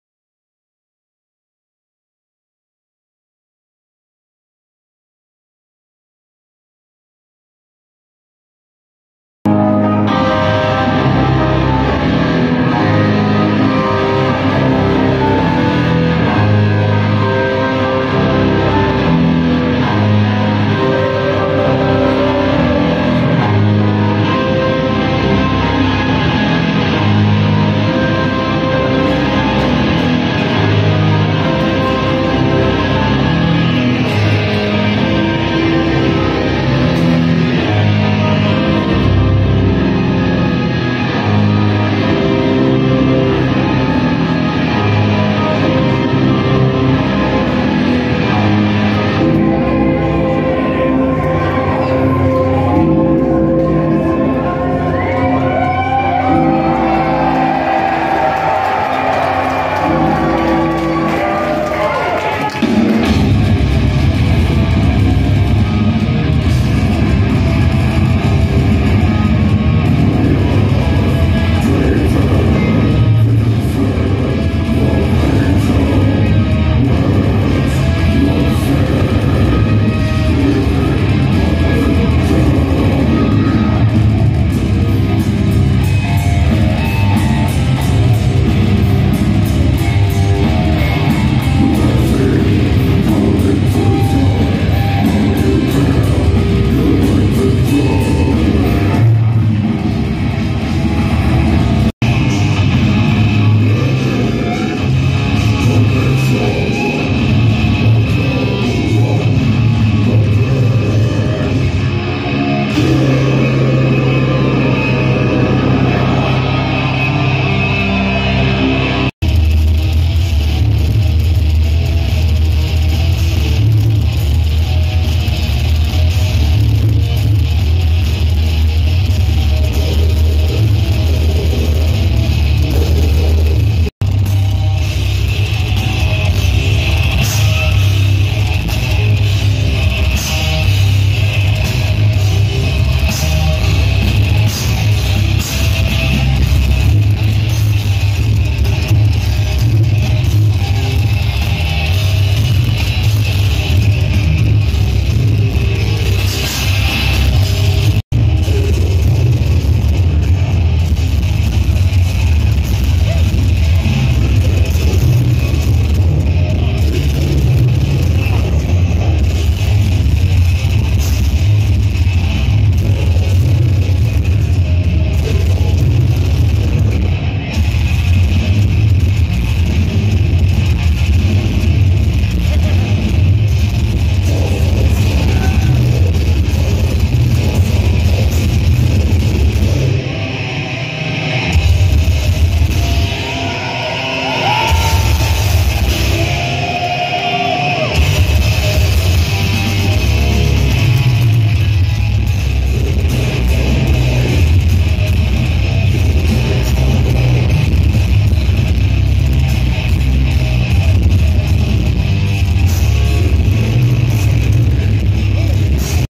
Imolation live @ the Agora in Cleveland OH 11/8/22
killer set I couldn't stand being boxed in by everyone so eventually erupted and started the pit it was awesome